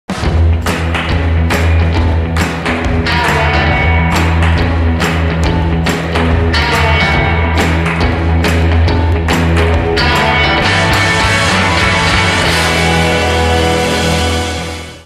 Lucchese boots video launching the new Spring 2014 western boots lines.
Lucchese boots handmade by artisans and craftsman since 1883 launched the spring 2014 collection with new western styles. Lucchese boots, handmade in Texas since 1883.